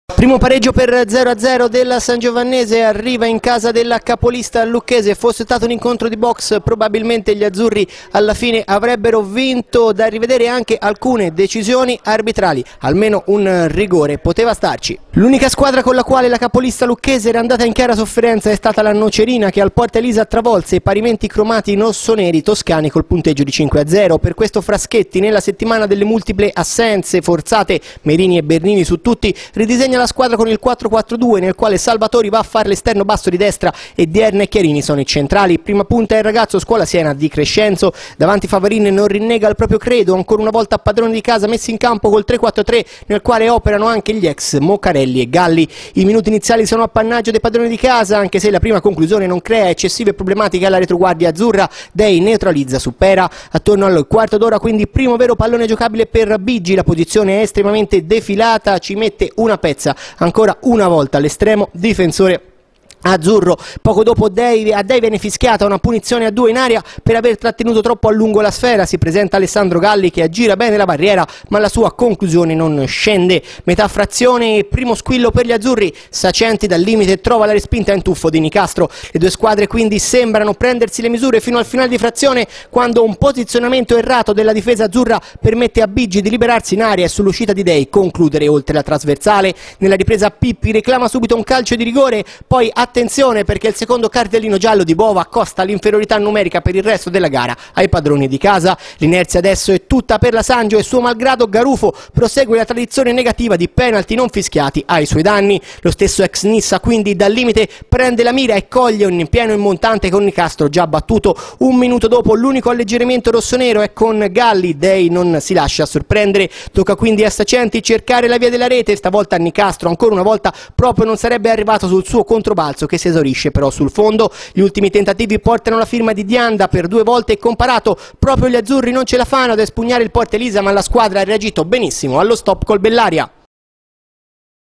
Ottava giornata Lega Pro Seconda Divisione Girone B Lucchese Sangio 0-0
Servizio di Alessandro Forni andato in onda su Teletruria e Valdarno Channel.
Buon pareggio degli azzurri in casa della capolista Lucchese. Gli azzurri possono recriminare per le tante occasioni avute (soprattutto il palo di Garufo) ed un paio di decisioni arbitrali dubbie.

IL TABELLINO:

SANGIOVANNESE (4-3-3): Dei; Dierna, Chiarini, Dianda, Salvatori (81' Tabacco); Bricca (A), Sacenti, De Cristofaro (A); Garufo, Raimondi (46' Pippi), Di Crescenzo (68' Comparato). A Disp.: Vaccarecci, Pelagatti, D'Ambrosio, Zane. All.: Fraschetti

LUCCHESE (4-3-3): Nicastro; Baldanzeddu, Bova (E), Lollini, Michelotti; Mariotti (A),Cheikh Merai, Mocarelli; Pera (58' Petri), Galli (66' Vannucci), Biggi (70' Scandurra). A Disp.: Lenzi, Krass, Carloto, Taddeucci. All.: Favarin

ARBITRO: Gavillucci di Latina coad. da Lunardon di Busto Arsizio e Piovera di Seregno.
NOTE: angoli 7-1